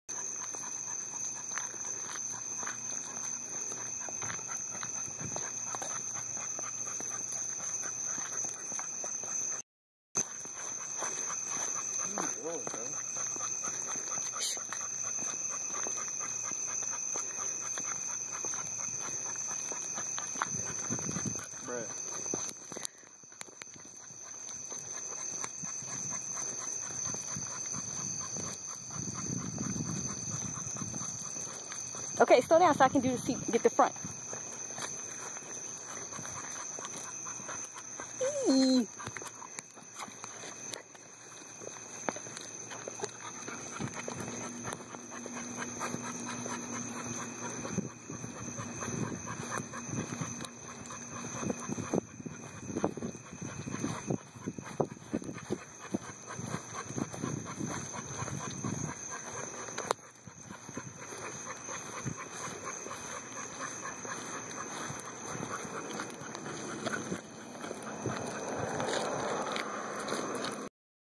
Pet Ego Comfort Wagon Stroller
Bought this for my 9year old pit ull. since he wasn't making the long walks as well as he use to.